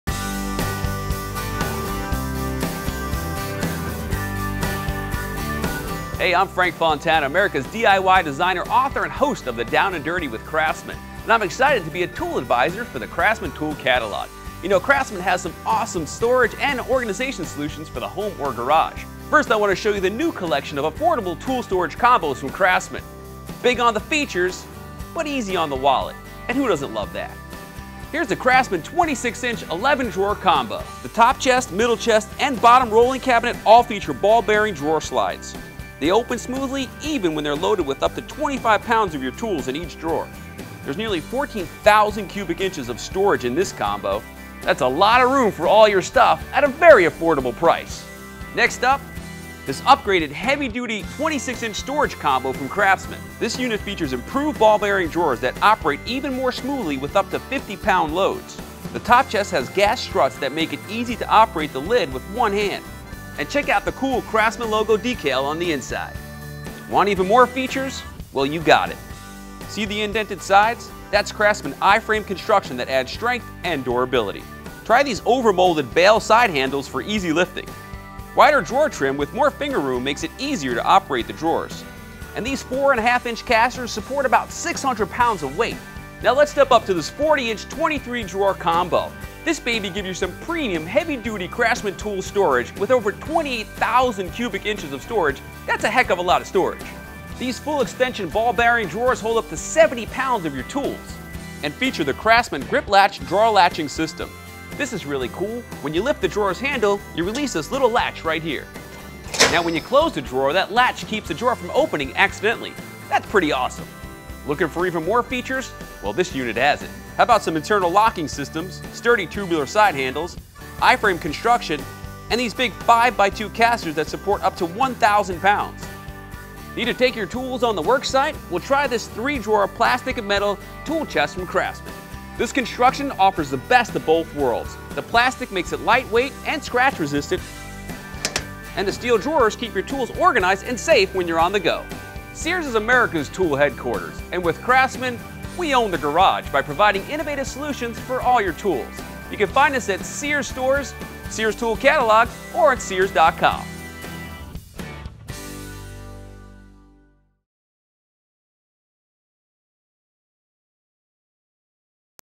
Craftsman: Frank Fontana
Video we produced for Craftsman Tools featuring DIY Network star Frank Fontana.